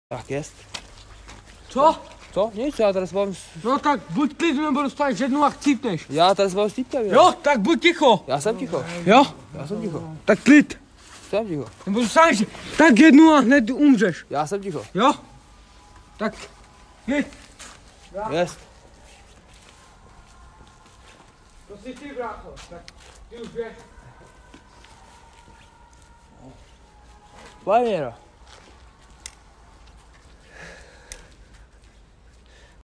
Novby - dostaneš jednu a umřeš
Twitch clip z kanálu Jirkos dostává zase vyhrožování na streamu